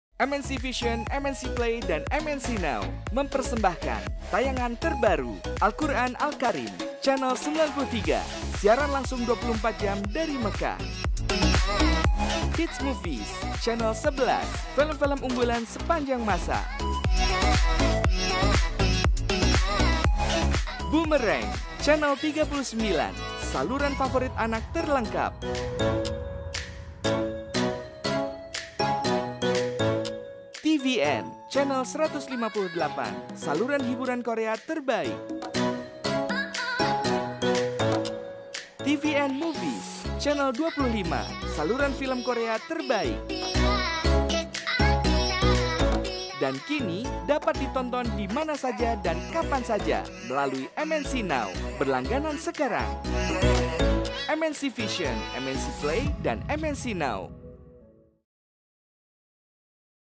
Penambahan Channel Baru MNC Vision, MNC Play dan MNC Now
MNC Vision, MNC Play, MNC Now mempersembahkan tayangan terbaru dan terbaik & Sobat Vision bebas pilih sesuai kesukaan & passion Kamu!

🎥Al Quran Al Kareem CH.93 siaran langsung 24Jam dari Mekkah
🎥Hits Movies CH.11 film- film unggulan sepanjang masa
🎥Boomerang CH.39 saluran favorit anak terlengkap
🎥TVN CH. 158 saluran hiburan Korea terbaik
🎥TVN Movies CH. 25 saluran film korea terbaik

Dan kini kamu juga bisa NONTON DIMANA SAJA & KAPAN SAJA melalui MNC Now.